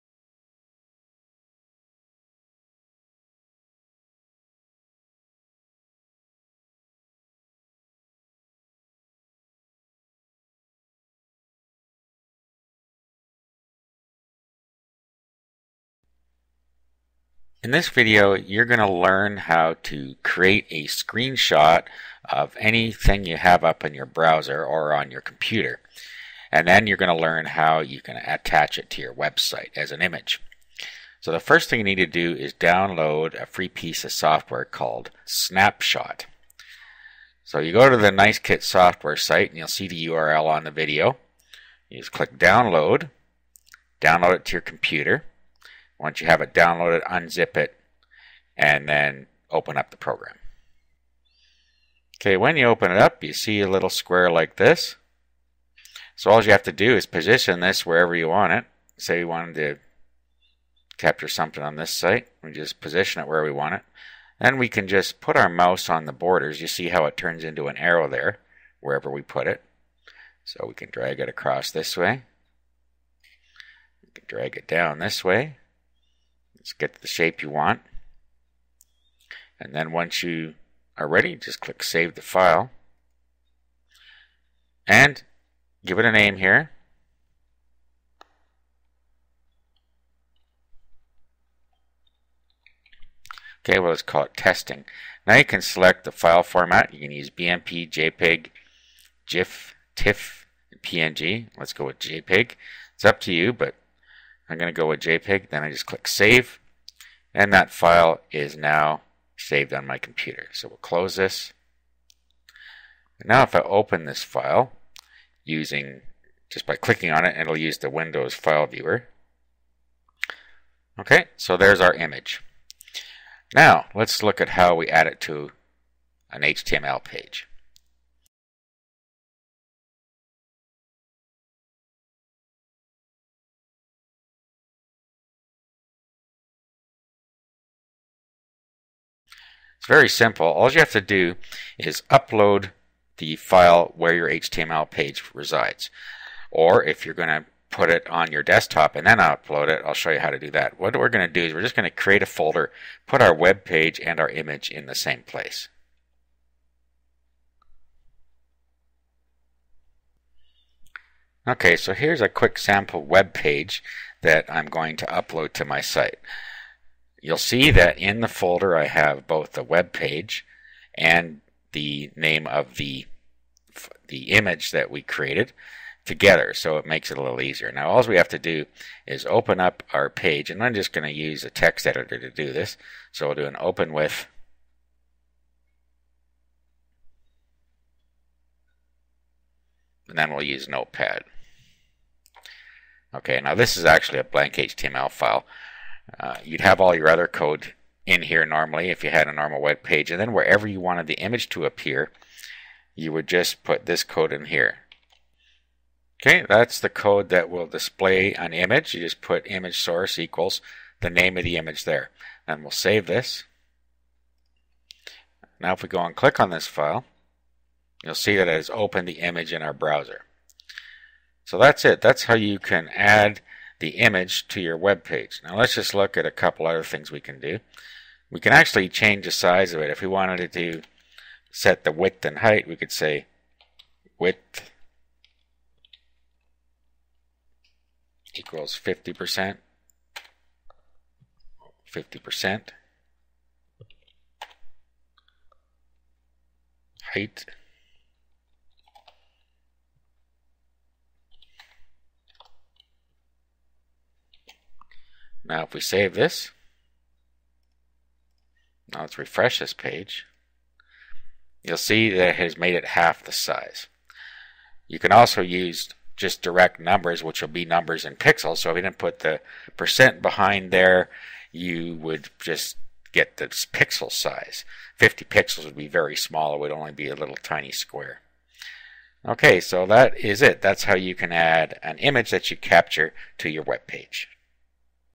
How To Capture Screen Images and Add Them to Your Website
Capture Images And Add Them To Your Website. View more video tutorial series from our official site here: -

Video Explaining How To Capture Screen Images and Add Them to Your Website. Capturing parts of pages to use in your webpages using free software.